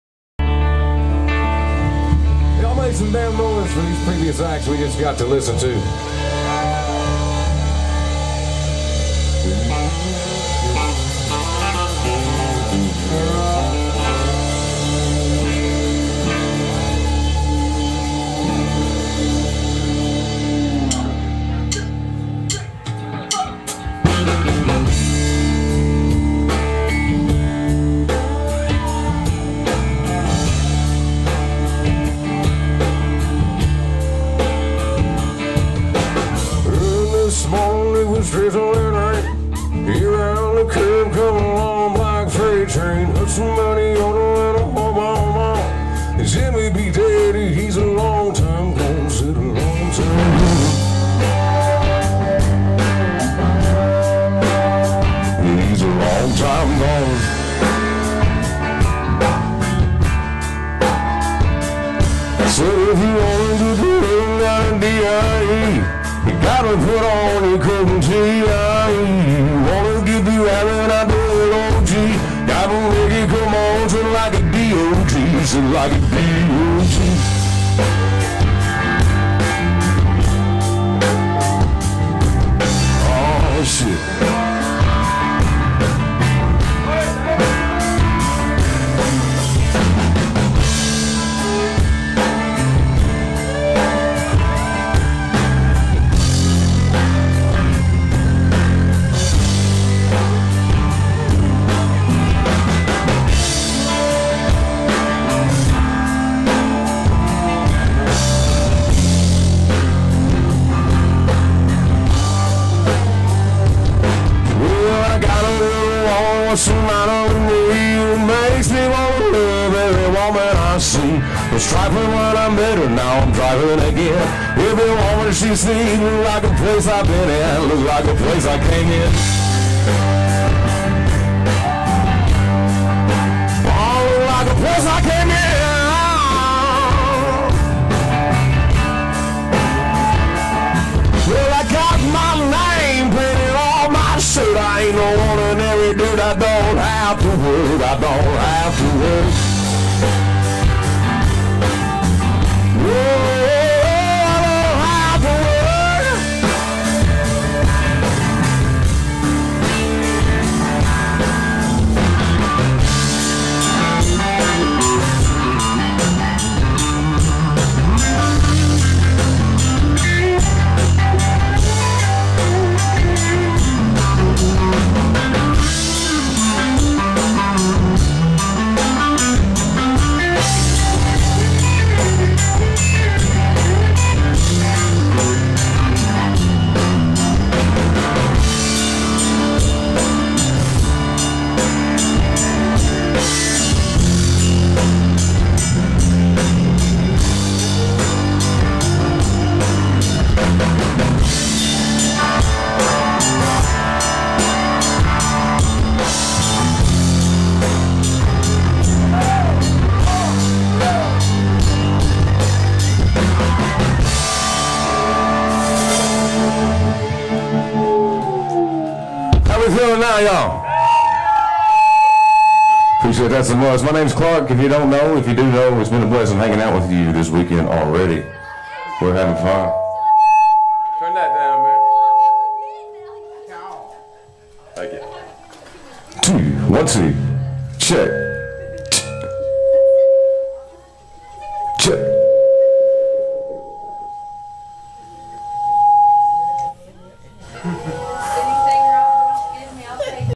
Clarke 2
RAWKIN' The River Sean Whiting and The Spinning Wheels Oct 3, 2020
Bones' Tavern at the Barn on the River